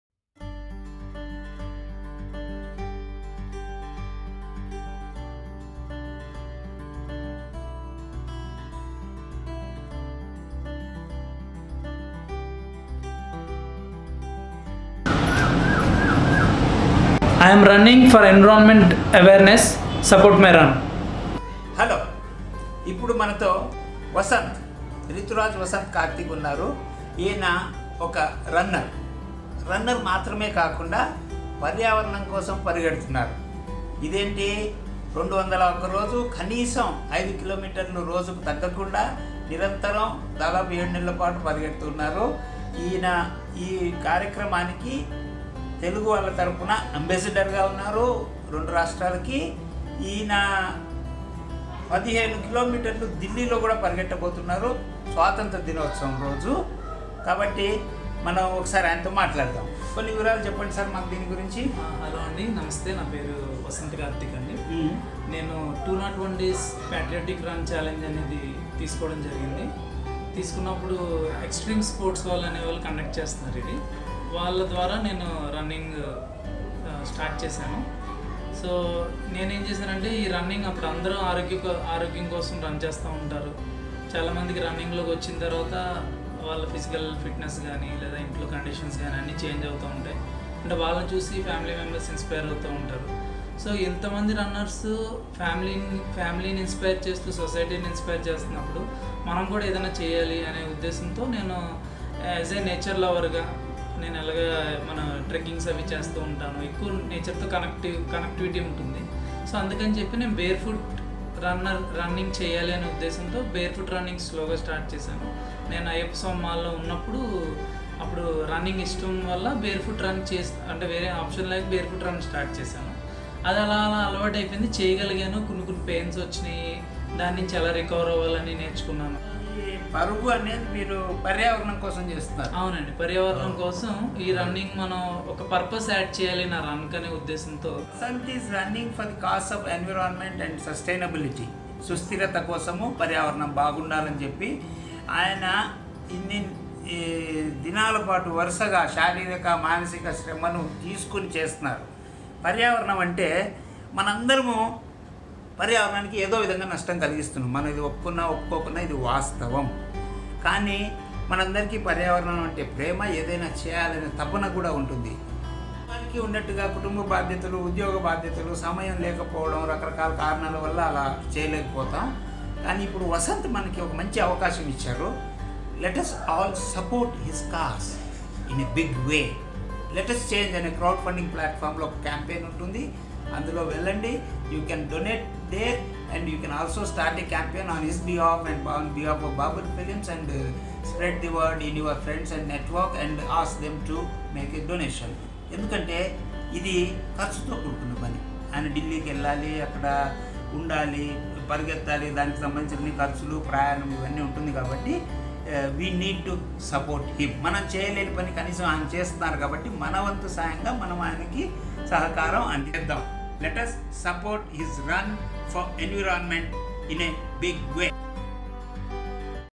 Run for Environment by Rituraj Vasanth Karthik
Rituraj Vasanth Karthik is running Patriotic run for 201 days that culminates on 15th August19 at New Delhi. He is raising funds to support his run and also for Environment Awareness. Let us support him in a big way!